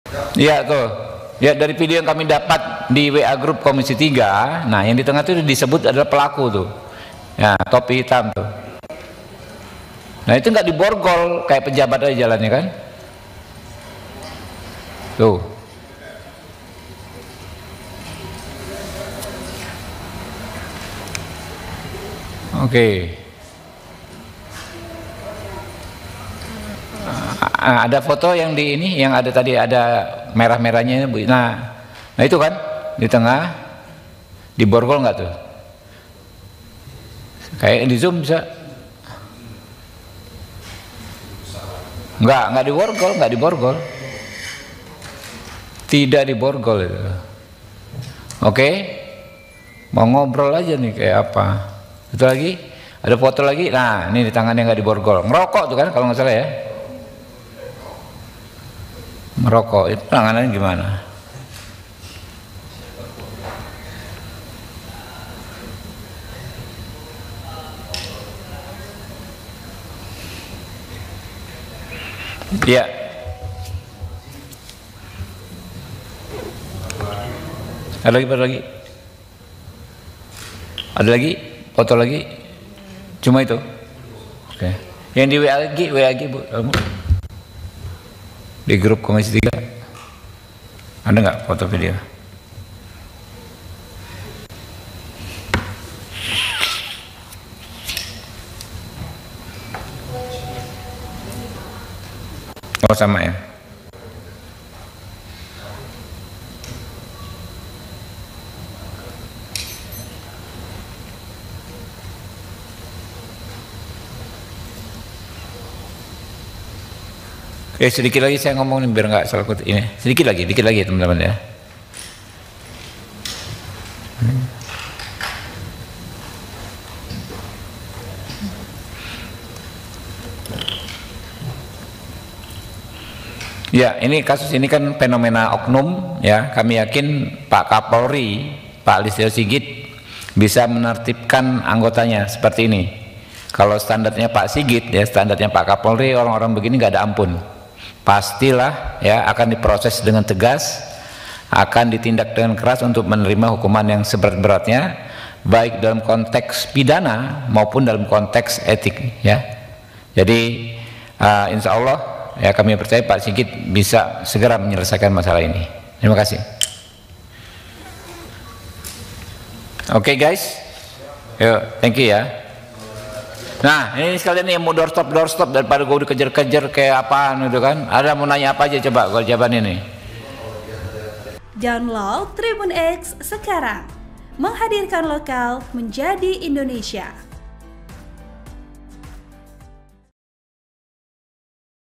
NADA TINGGI KOMISI III DPR RI! Geram Lihat AKP Dadang Tak Diborgol, Yakin Kapolri Tak Beri Ampun!
TRIBUN-MEDAN.COM - , Komisi III DPR RI meradang saat melihat proses penyerahan diri AKP Dadang Iskandar usai melakukan penembakan kepada AKP Ulil Ryanto.

Wakil Ketua Komisi III DPR RI Habiburokhman menunjukkan video penyerahan diri AKP Dadang yang dikawal Propam Polda Sumbar.

Habiburokhman geram lantaran AKP Dadang tak diborgol dan terkesan santai saat diperiksa.

Komisi III DPR RI yakin Kapolri Jenderal Listyo Sigit Prabowo tak beri ampun oknum polisi tersebut.

Editor Video : Fanry Maulana